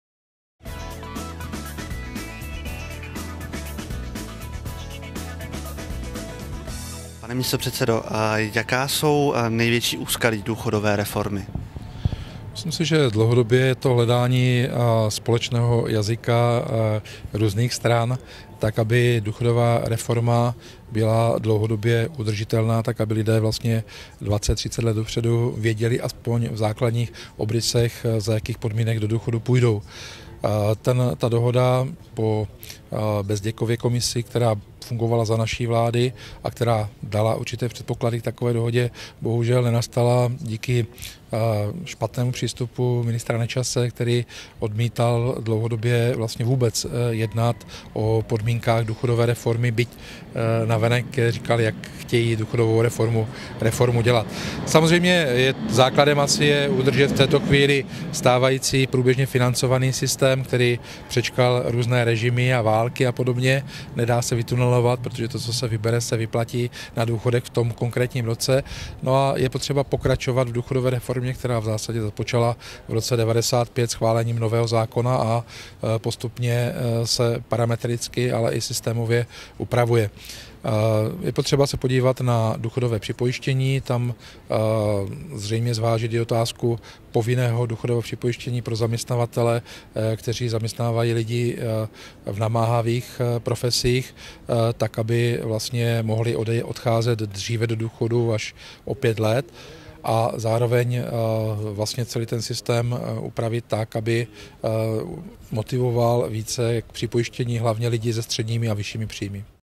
Zdeněk Škromach: V důchodové reformě musíme pokračovat
Dlouhodobým problémem v případu důchodové reformy je podle místopředsedy sociální demokracie Zdeňka Škromacha hledání společného jazyka politických stran.

Hlavně tedy v tom, aby byla důchodová reforma dlouhodobě udržitelná a lidé alespoň dvacet let dopředu věděli, za jakých podmínek půjdou do důchodu. „Dohoda která fungovala za naší vlády a poskytla určité kroky k této dohodě, bohužel nenastala díky špatnému přístupu ministra Nečase. Ten dlouhodobě odmítal jednat o podmínkách důchodové reformy, říká Zdeněk Škromach. Podle něj je základem udržet současný průběžně financovaný systém a pokračovat v důchodové reformě.